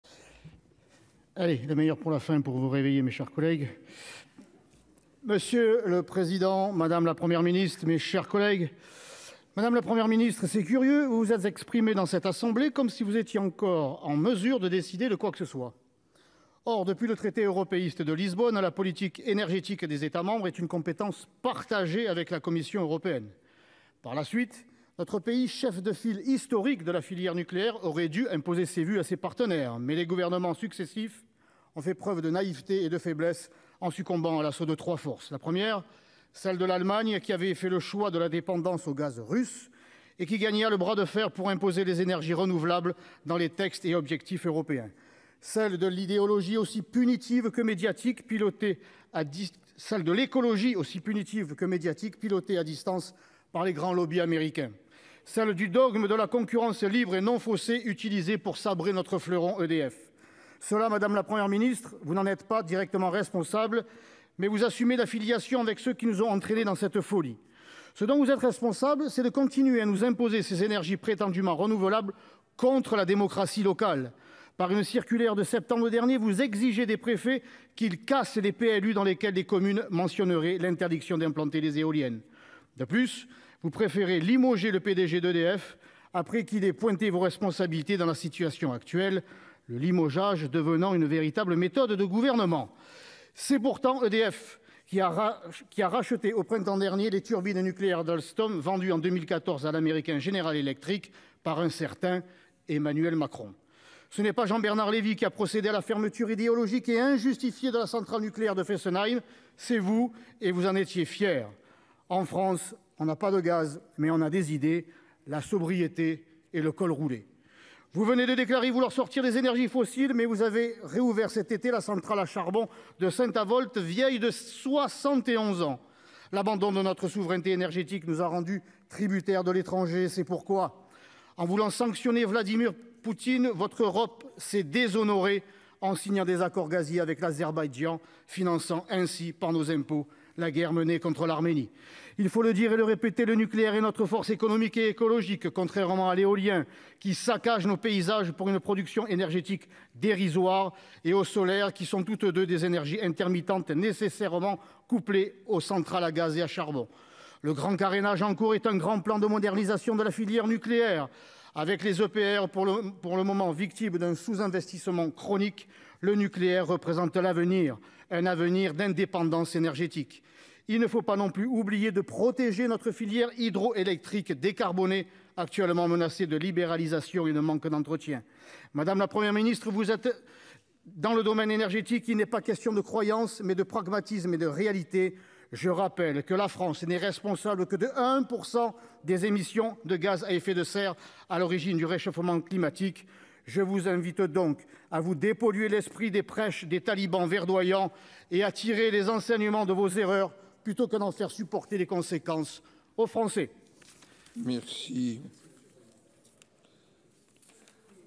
La France n’est responsable que de 1% des émissions de gaz à effet de serre | Stéphane Ravier
Le nucléaire est notre force économique et écologique, grâce auquel la France n’est responsable que de 1% des émissions de gaz à effet de serre.
L’éolien saccage nos paysages pour une production énergétique dérisoire et est, comme le solaire, une énergie intermittente nécessairement couplée aux centrales à gaz et à charbon.